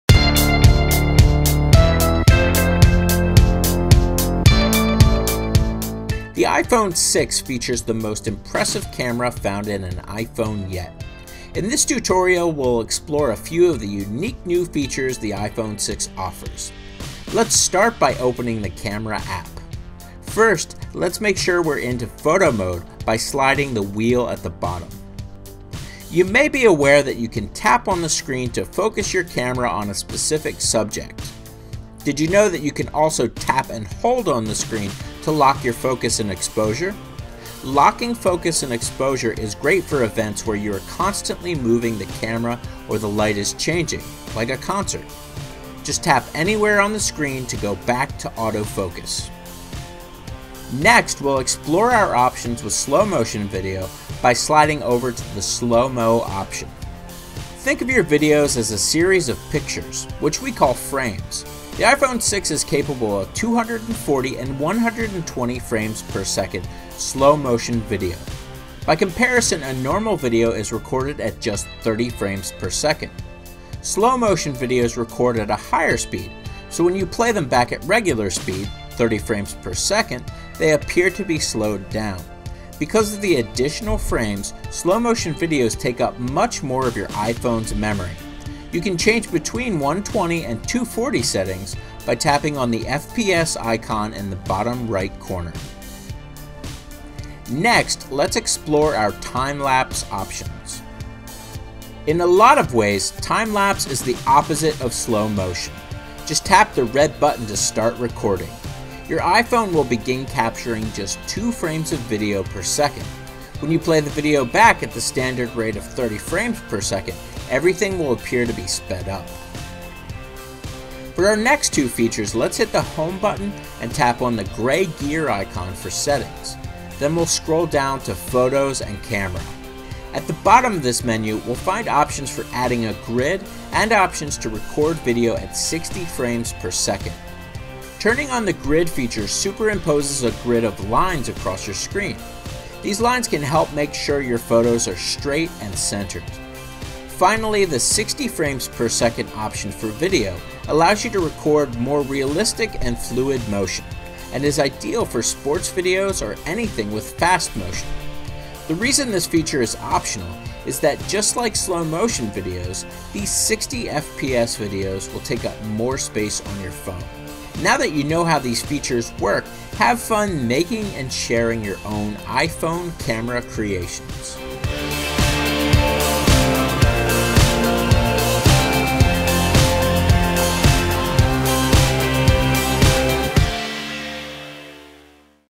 iPhone 6 Camera Features Tutorial
We take you through the great features that come with the iPhone 6 camera, including super slow mo 240 fps, adding grids to your camera, focus lock and a lot more.